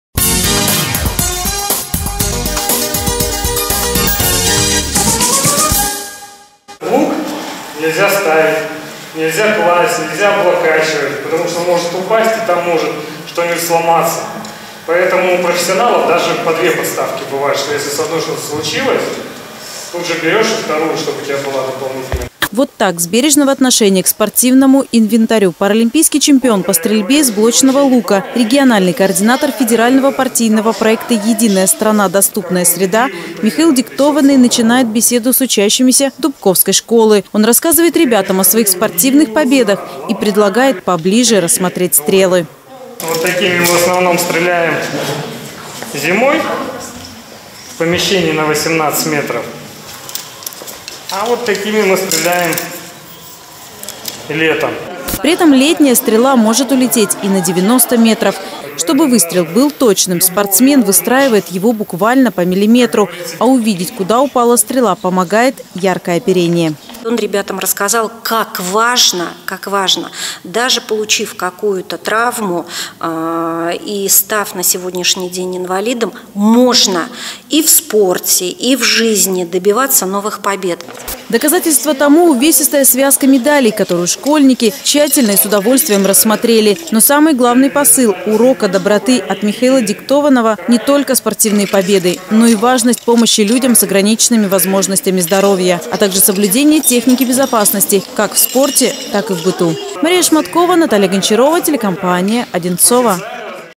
Уроки доброты Михаила Диктованного
Паралимпийский чемпион по стрельбе из блочного лука, региональный координатор федерального партийного проекта «Единая страна – доступная среда» Михаил Диктованный провел «Урок доброты» в Дубковской школе «Дружба».